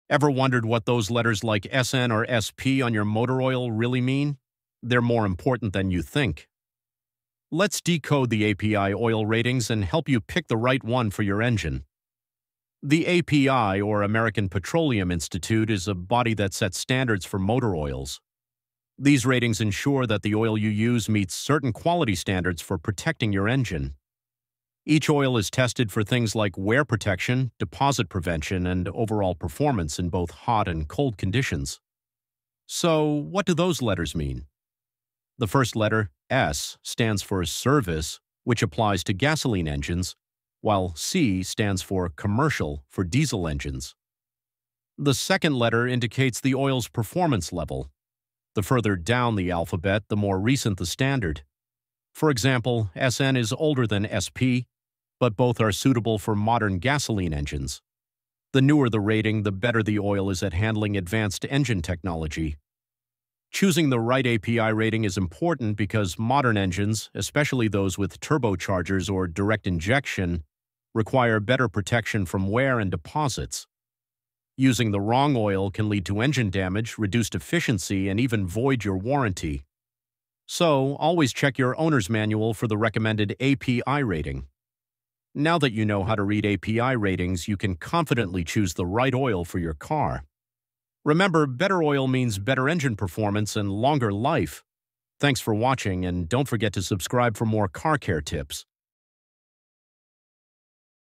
Understanding API Oil Ratings: How to Choose the Right Oil for Your Car
🛢️ Confused about API oil ratings? You’re not alone! In this video, we’ll walk you through what those letters like “SN” and “SP” really mean and why they’re crucial for your car's engine. 🚗💡 Learn how to choose the right motor oil based on your car’s needs, whether it’s for a new engine or an older one. Picking the correct american petroleum institute (API) rating can help improve engine performance and longevity. 🔧 Ready to find out which oil is right for your car? Watch now!